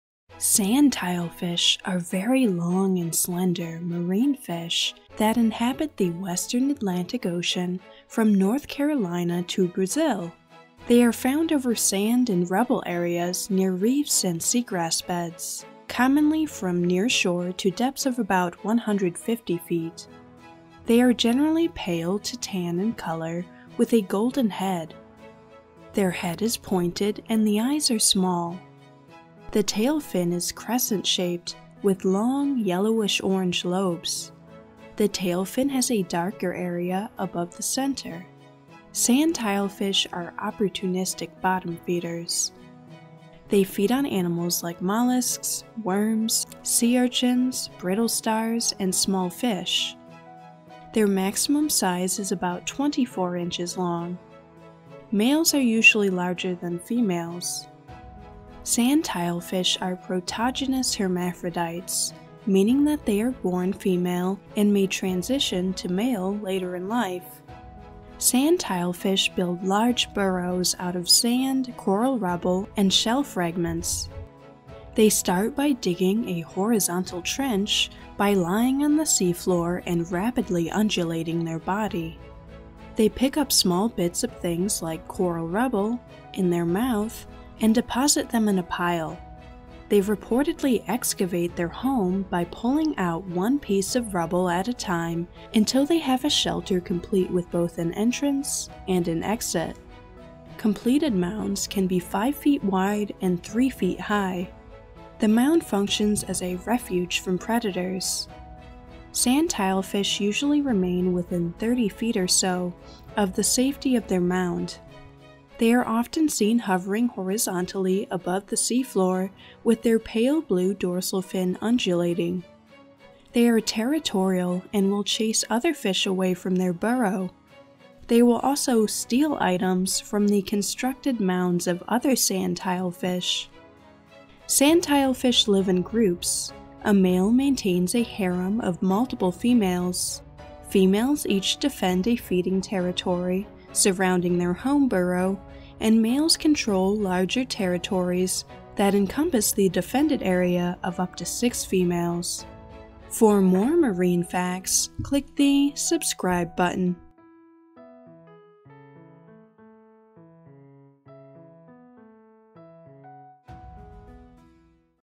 Facts: The Sand Tilefish
Quick facts about these adorable, burrowing fish! The sand tilefish (Malacanthus plumieri). Sand tilefish facts!

Snyderman, Marty, and Wiseman, Clay. Guide to Marine Life: Caribbean, Bahamas, Florida. United States, Aqua Quest Publications, 1996.

Stoops, PJ, and Stoops, Benchalak Srimart. Texas Seafood: A Cookbook and Comprehensive Guide. United States, University of Texas Press, 2019.

Robins, C. Richard, and Ray, G. Carleton. A Field Guide to Atlantic Coast Fishes of North America. United States, Houghton Mifflin, 1986.

Snyder, David B., and Burgess, George H.. Marine Fishes of Florida. United States, Johns Hopkins University Press, 2016.

McDougall, Peter, et al. Reef Smart Guides Palm Beach, Florida. United Kingdom, Mango Media, 2019.
NOAA Photo Library
By Fernando Herranz Martín - This image is uploaded as image number 7571 at Animalandia at Educa Madrid, a source of photographs of animals.This tag does not indicate the copyright status of the attached work. A normal copyright tag is still required. See Commons:Licensing., GPL,